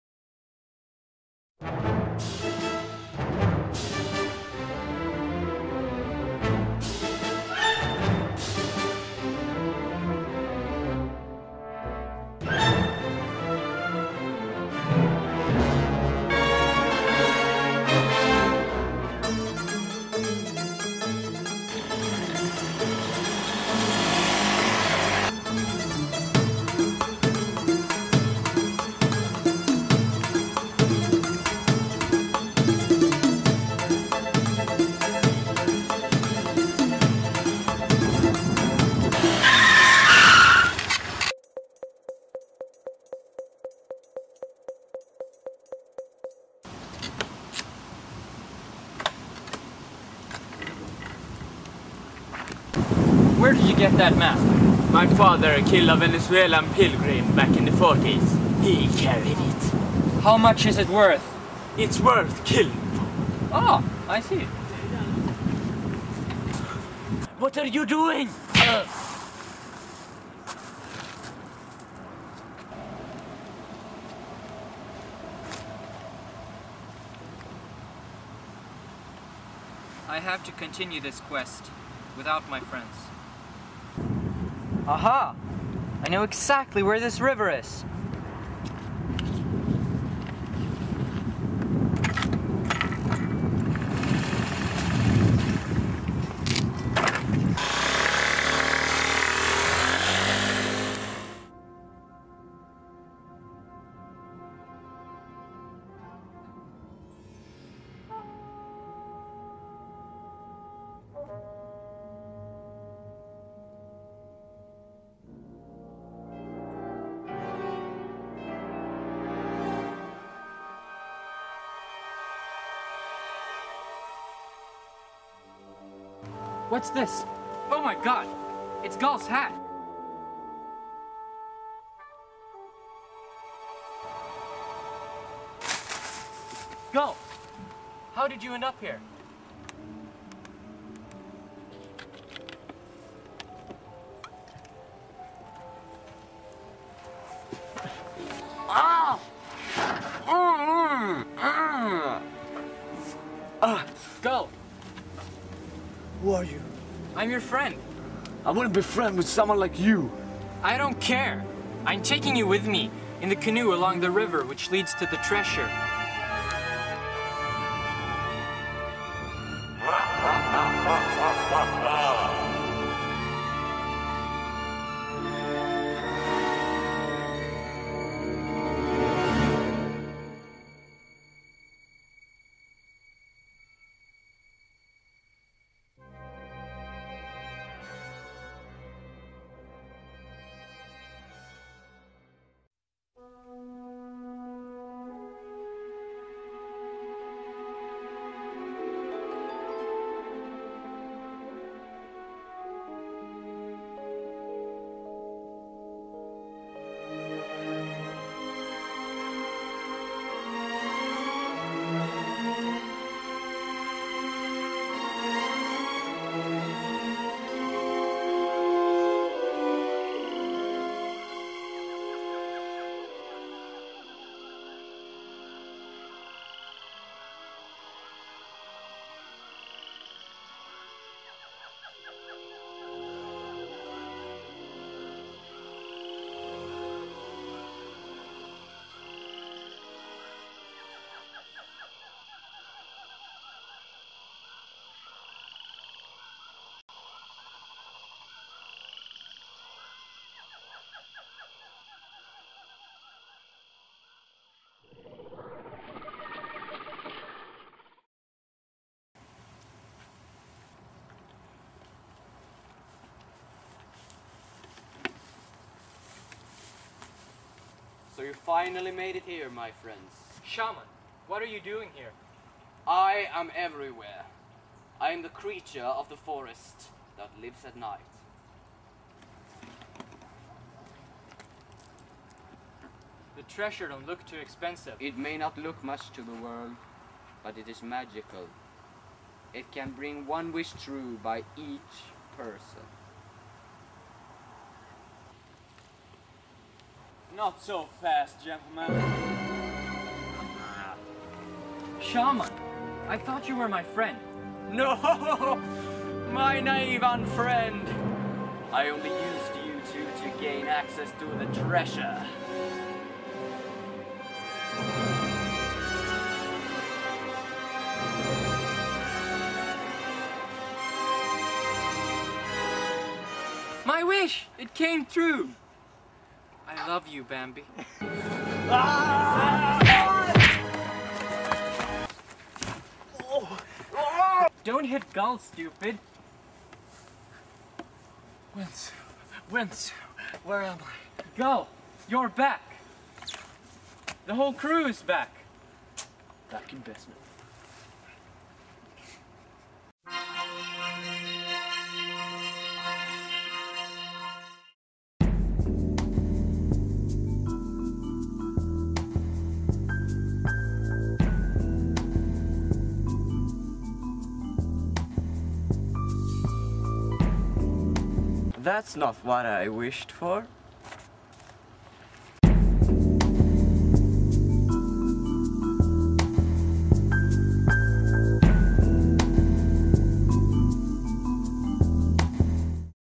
The Quest (Episode 4)
The exciting finale of the Quest! Vince finds a treasure map and goes to Venezuela! Features wonderful nature scenes from a canoe and grand action!